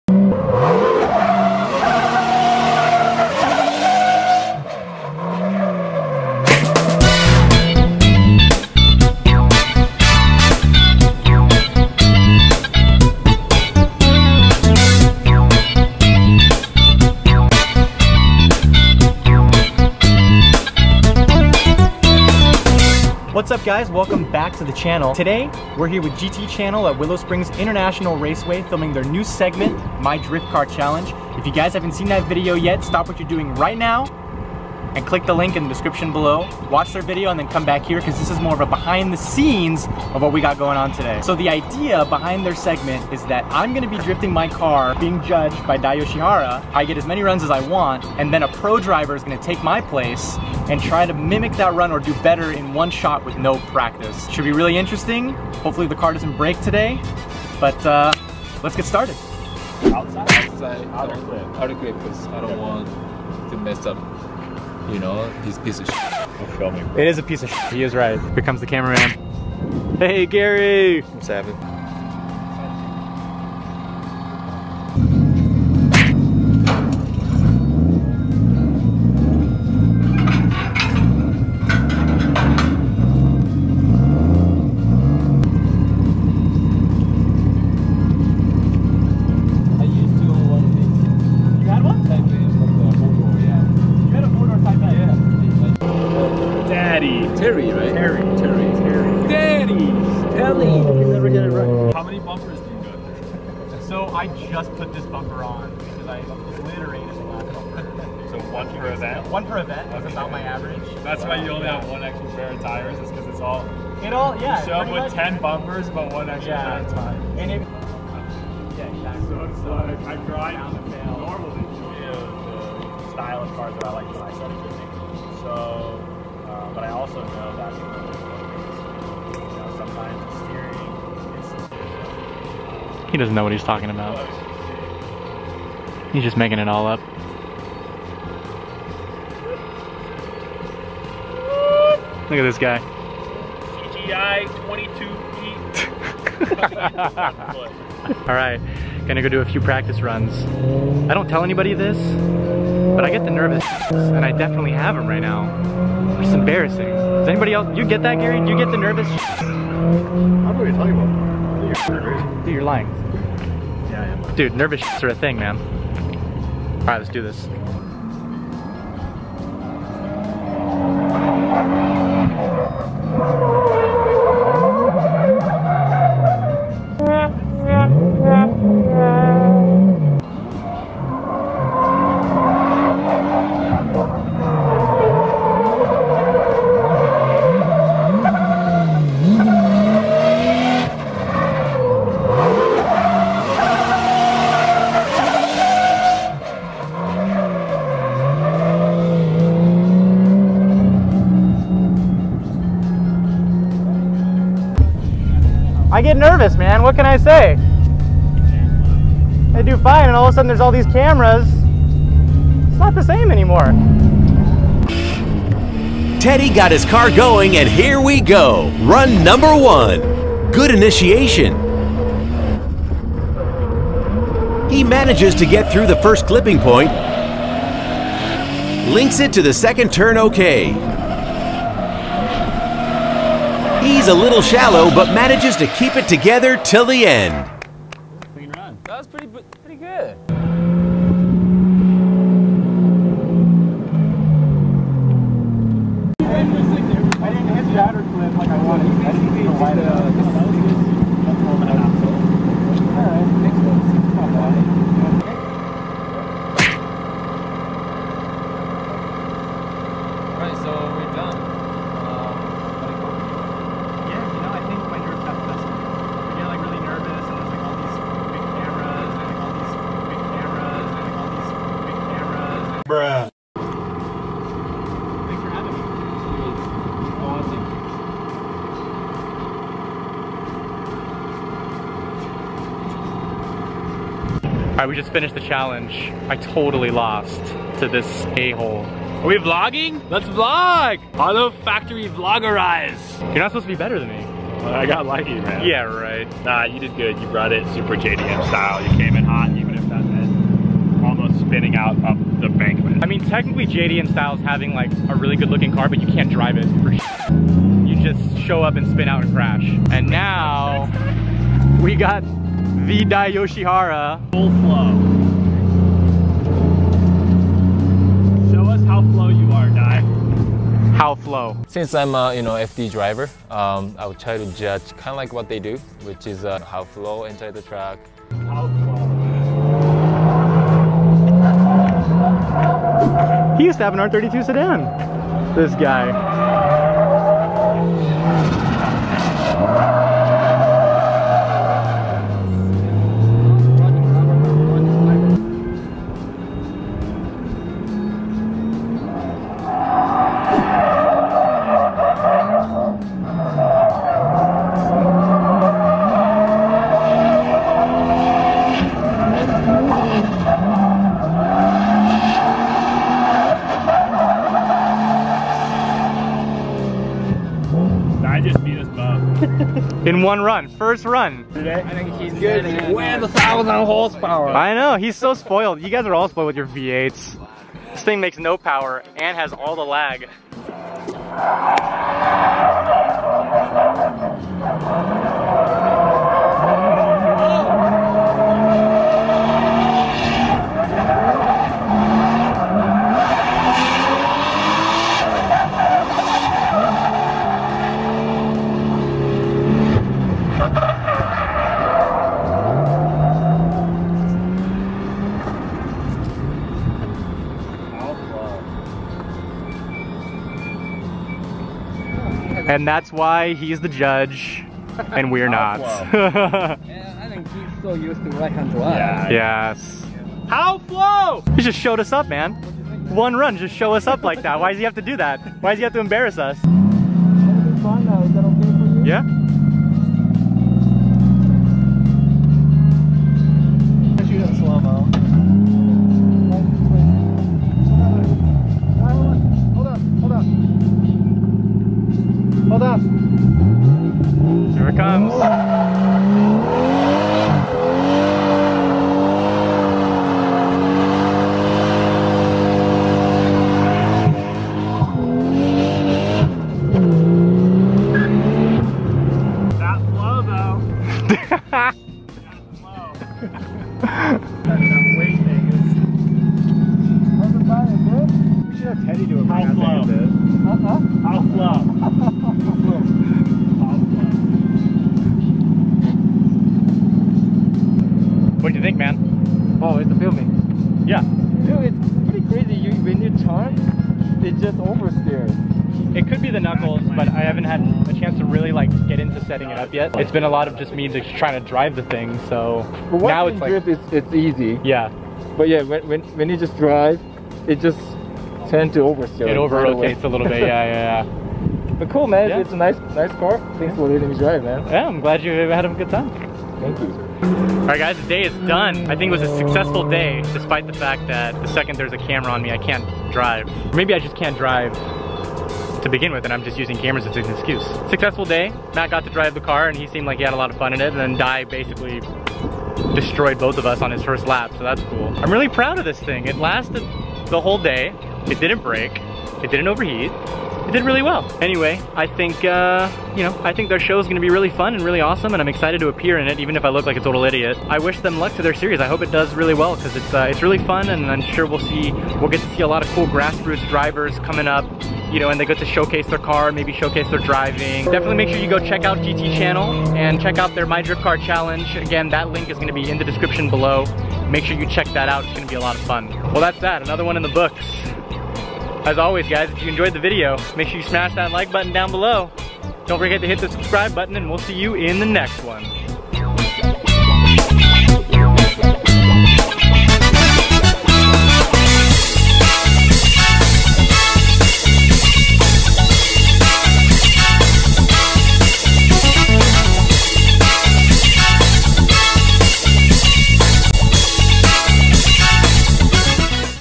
My Drift Car Challenge VLOG - Matt Powers/Dai Yoshihara DRIVE MY CAR!
We appeared on GT Channel's "My Drift Car Challenge"! Today we go behind the scenes of the shoot at Willow Springs Raceway.

Hit us with any questions you might have, or let us know what you'd like to see in future videos!

Turn post notifications on so you don't miss our next video!

Our radiators and oil coolers